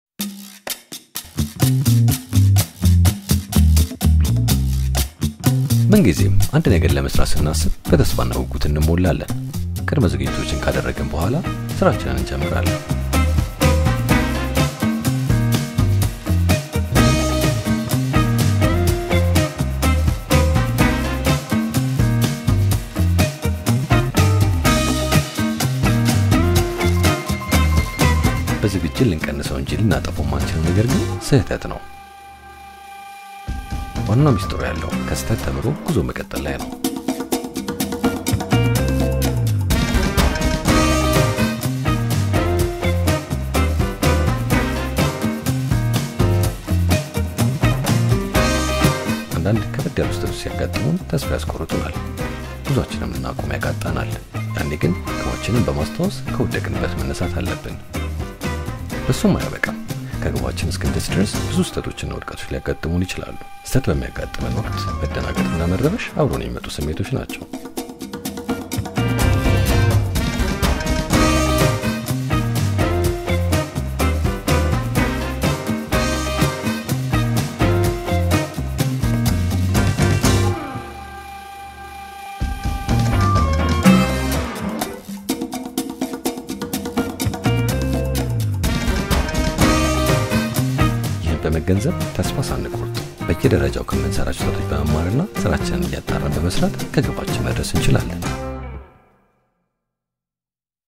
"በመንገዳችን ላይ ብዙ ስህተቶች እና ውድቀቶች ያጋጥሙናል!"
🎥 Prepare to be moved by a powerful message in this short, emotionally charged video. In just a few minutes, you'll witness the journey of a determined individual who faces countless obstacles but never gives up.

This inspirational story highlights the essence of resilience and the unwavering spirit to conquer adversity. From repeated setbacks to the ultimate triumph, it's a testament to the human capacity for perseverance.

keep pushing forward, no matter how tough the path may seem.